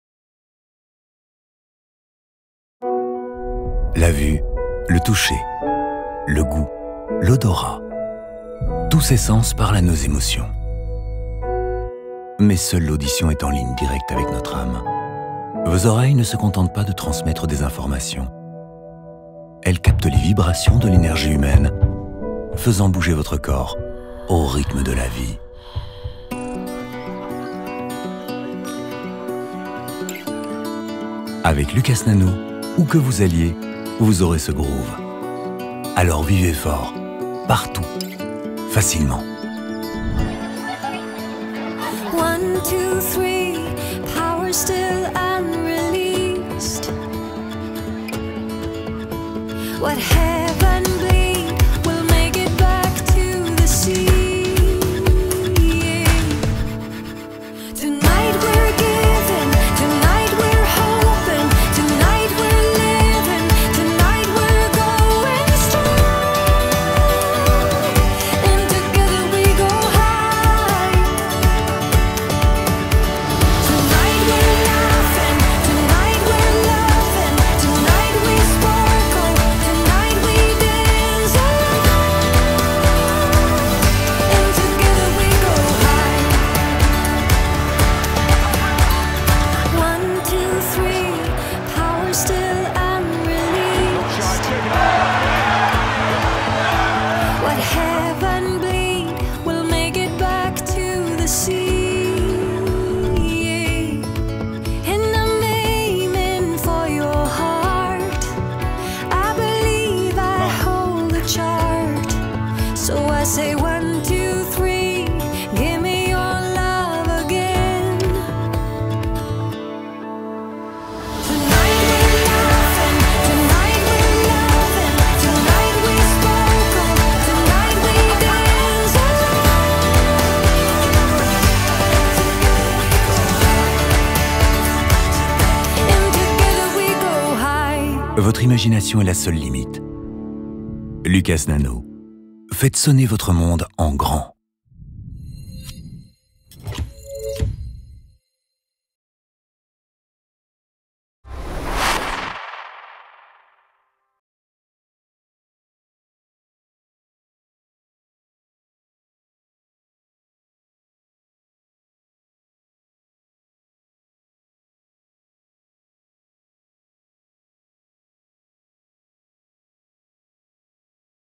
HK Lucas Nano 302-305FX-605FX - La nouvelle génération de sonorisation portable (La Boite Noire)
La sonorisation portative HK Audio Lucas Nano révolutionne la façon d'amplifier le son avec une série de système compactes et polyvalents.